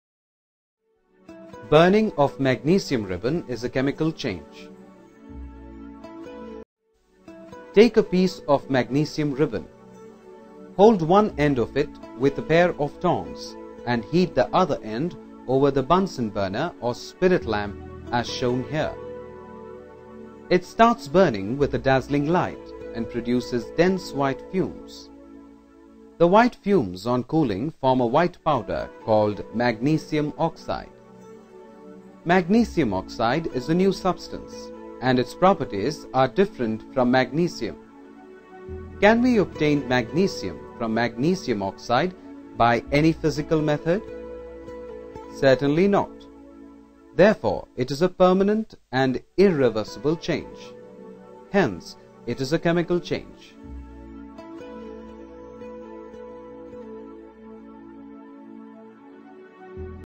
Burning Magnesium Ribbon
Magnesium is one of the most common  alkaline-earth metals in the Earth's crust.  In its pure form, it is silvery white, and relatively soft.  In its raw form it burns in air with white light, because of this property, it is used in fireworks.